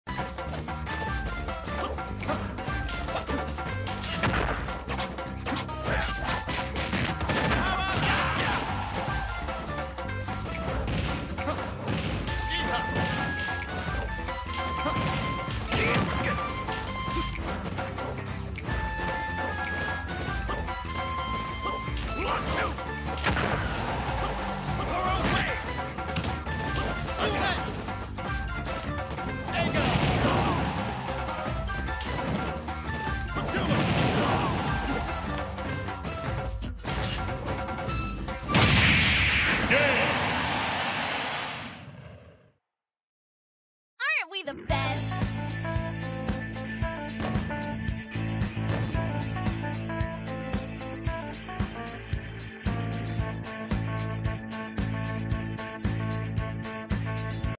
[No Distractions] Joker vs. Terry
Joker vs. Terry in a one stock battle of pure skill on the stage Balloon Fight.